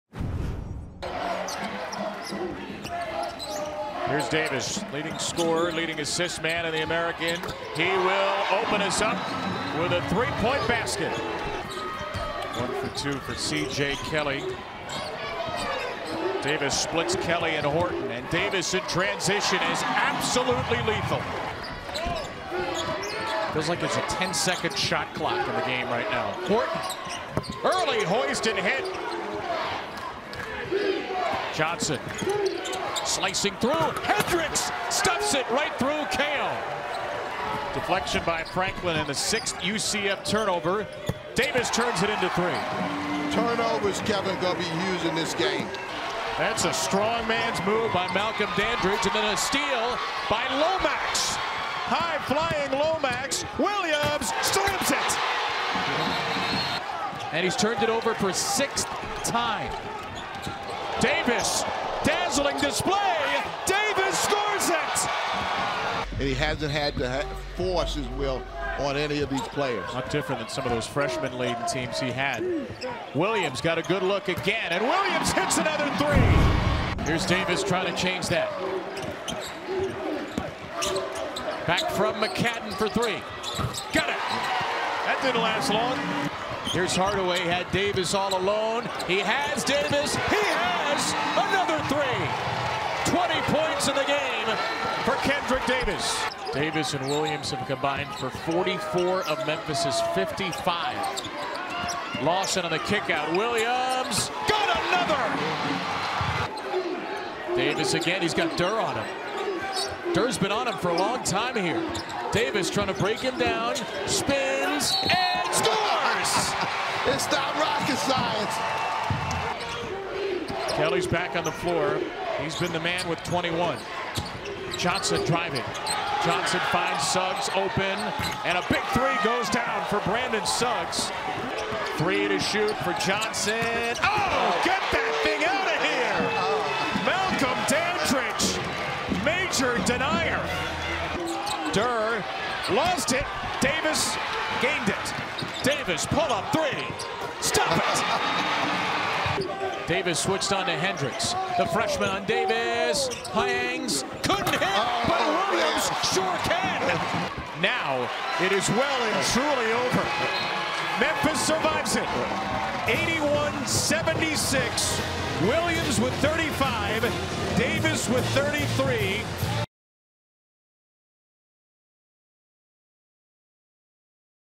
American MBB Championship: #7 UCF vs #2 Memphis (Quarterfinals) Game Highlights
Kendric Davis and DeAndre Williams combined for 68 points as No. 2 seed Memphis defeated seventh-seeded Memphis, 81-76, in the quarterfinals of the American Athletic Conference Men’s Basketball Championship Friday, March 10 at Dickies Arena in Fort Worth, Texas.

With the win, the Tigers (24-8) will face the winner of Wichita State/Tulane in the semifinals on Saturday, March 11 at 5 p.m. ET/4 p.m. CT.